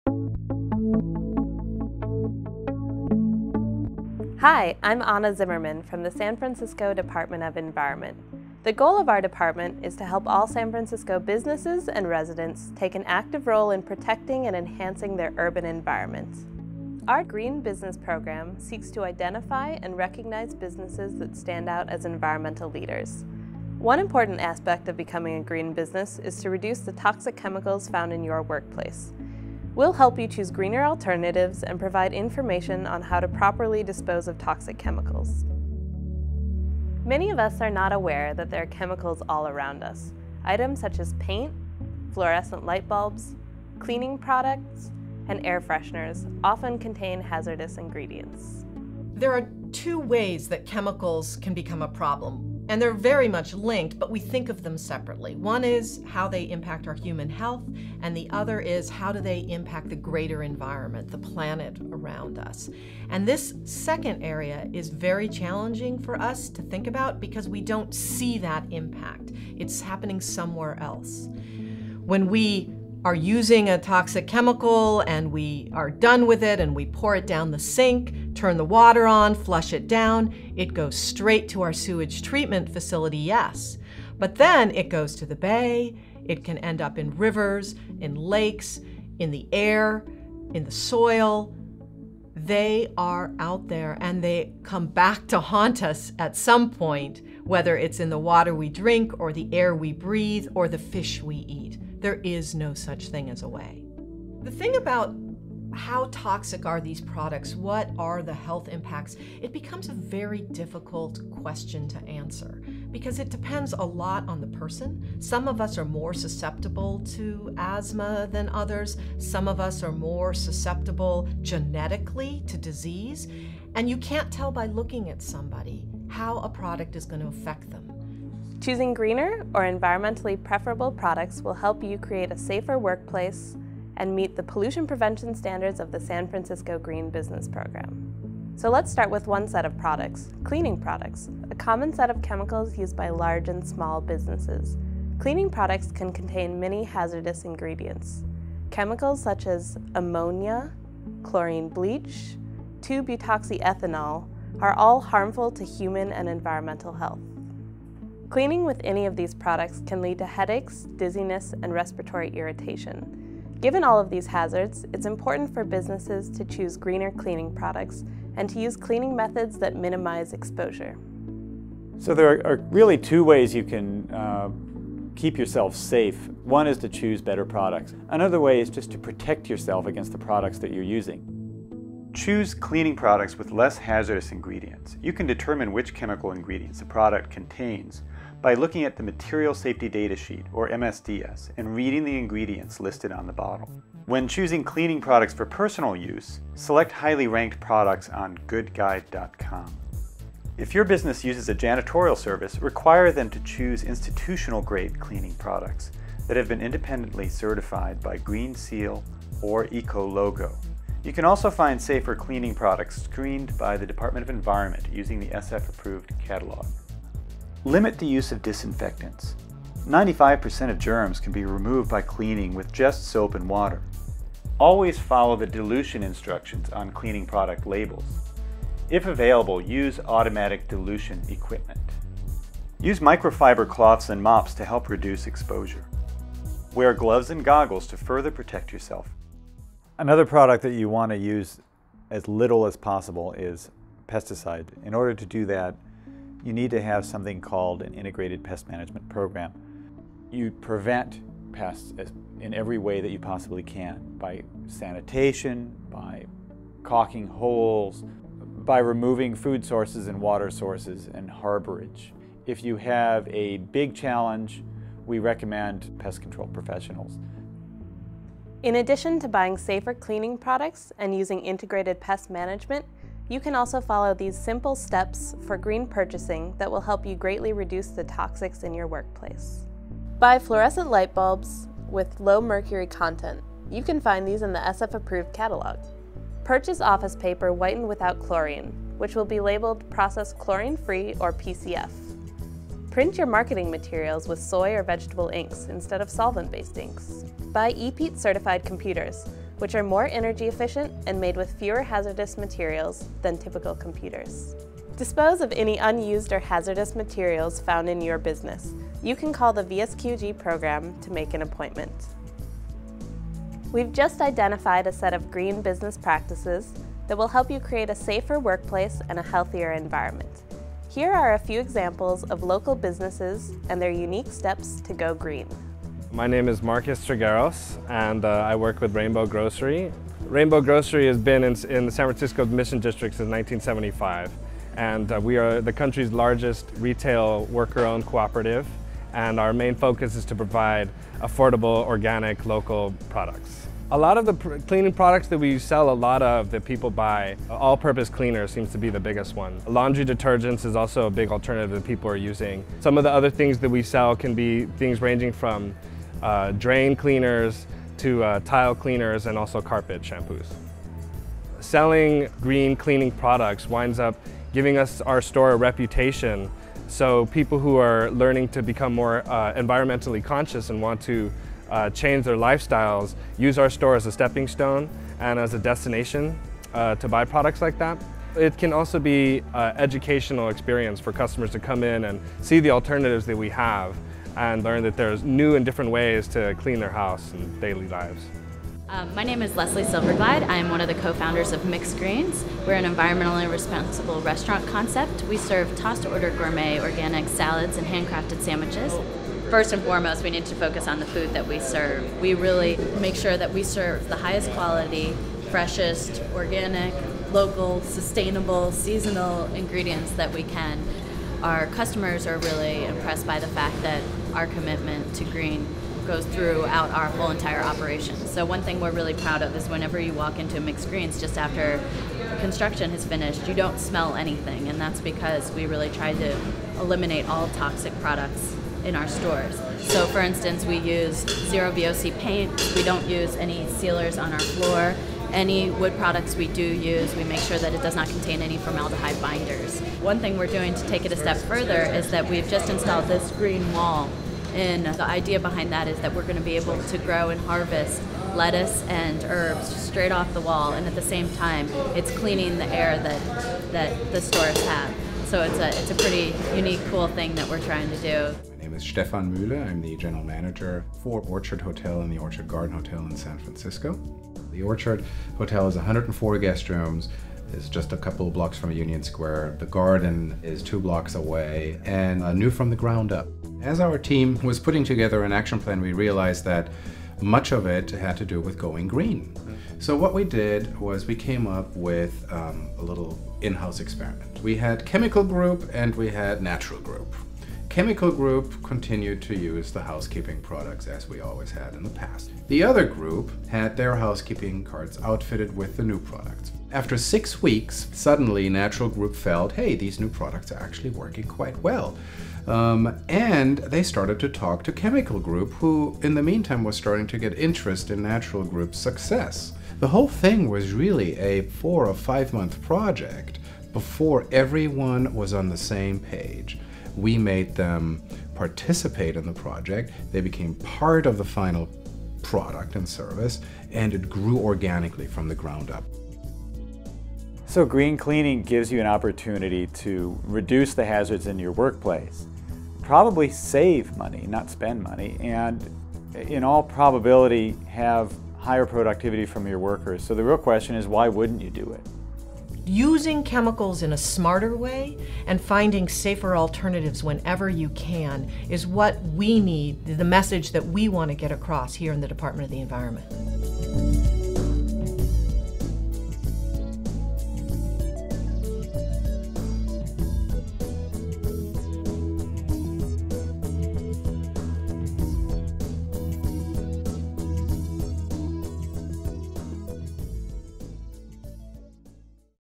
SF Green Business - Guide to Pollution Prevention
The SF Green Business guide to pollution prevention for a healthier and safe work environment.